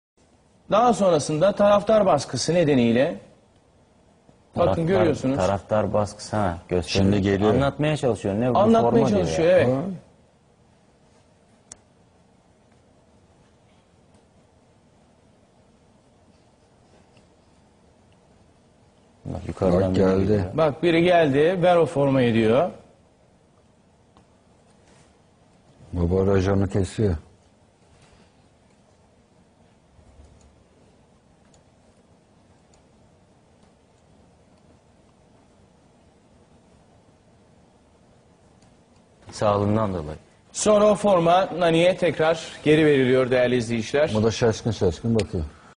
Nani'nin Formasını Alan Trabzonspor Taraftarı vs Maganda
Avni Aker'de oynanan 4-0 Fenerbahçe'nin üstünlüğüyle biten maçta, nani'nin formasını verdiği taraftara trabzonlular saldırıyor ve adam formayı geri göndermek zorunda kalıyor

not: kanalımıza destek olmak amacıyla reklamlara tıklarsanız seviniriz. iyi seyirler